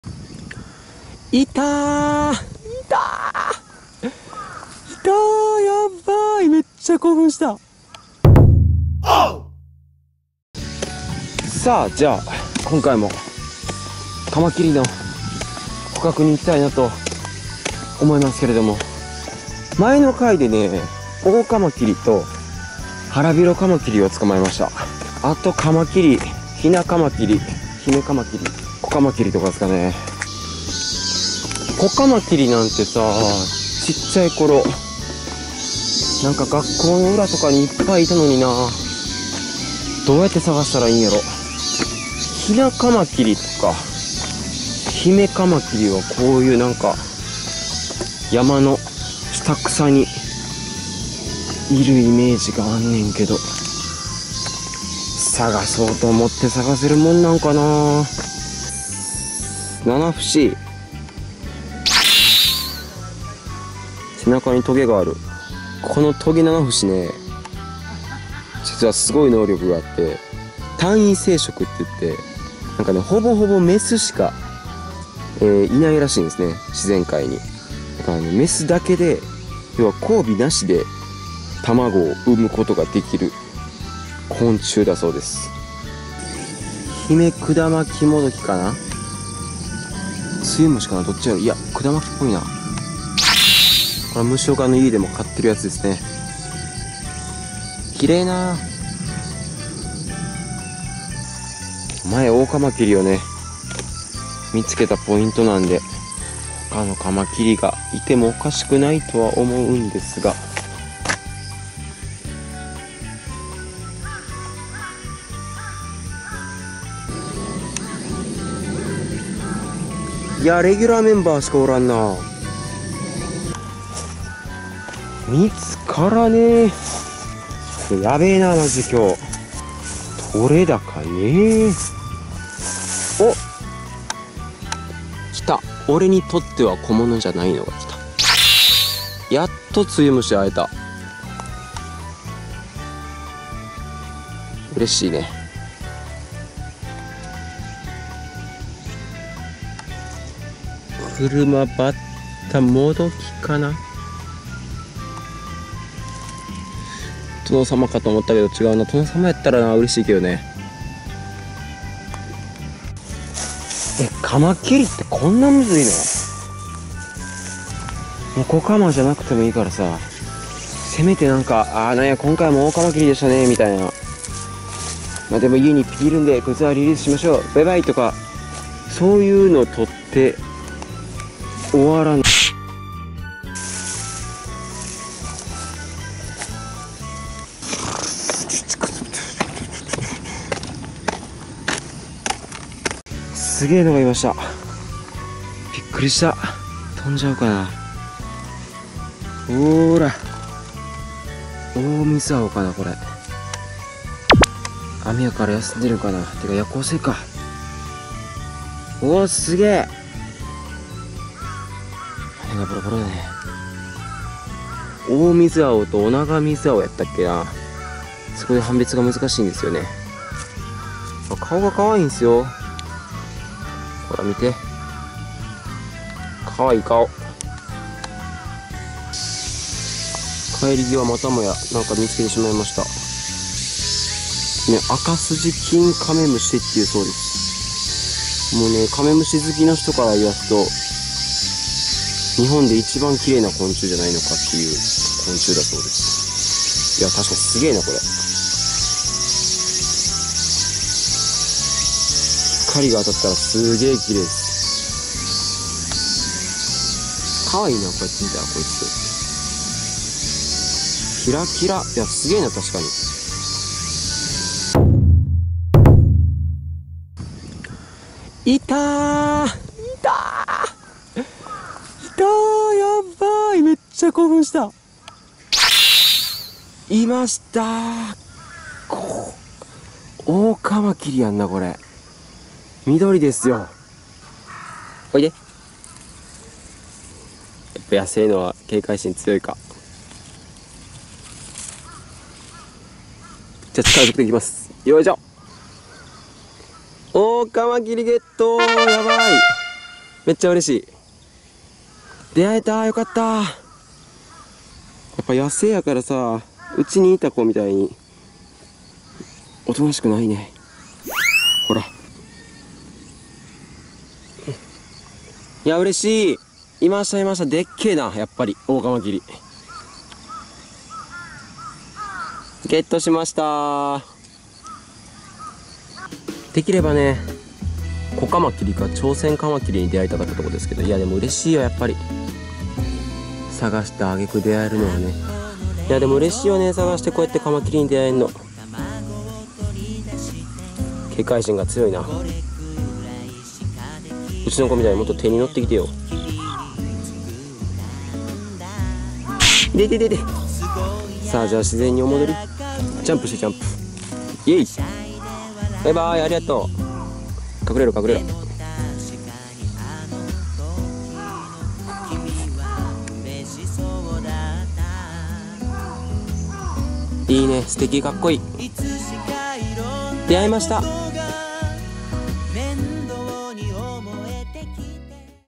【カマキリ採集】秋のオオカマキリ採集！Part2
コカマキリやチョウセンカマキリなど、
色んな種類のカマキリに出会いたいのですが、なかなか出会えず彷徨って、諦めて帰りかけたその時...!?

【訂正】2:33「クルマバッタモドキ」→「クルマバッタ」
ご指摘いただきましたm(_ _)m

・トゲナナフシ
・ヒメクダマキモドキ
・アカボシゴマダラ
・イチモンジセセリ
・ツユムシ
・オオミズアオ
・アカスジキンカメムシ
・オオカマキリ